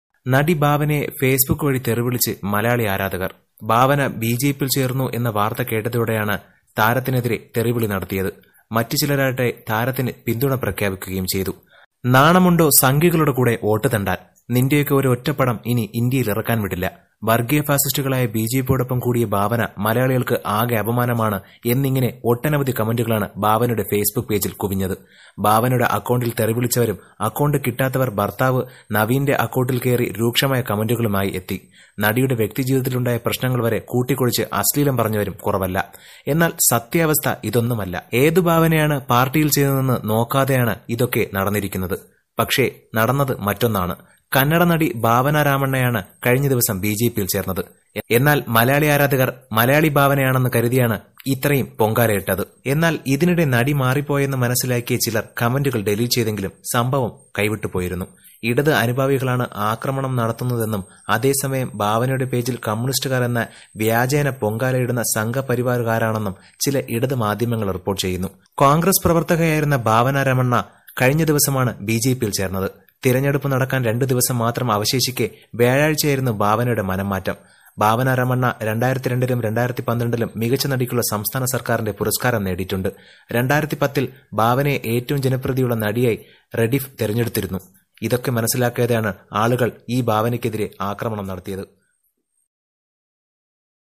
നടി ഭാവന ബിജെപിയിൽ ചേർന്നു തെറിവിളിയുമായി ആരാധകർ | bhavana join bjp party | bhavana angry
ഈ വീഡിയോ മുഴുവനും കാണാൻ ആഗ്രഹിക്കുന്നു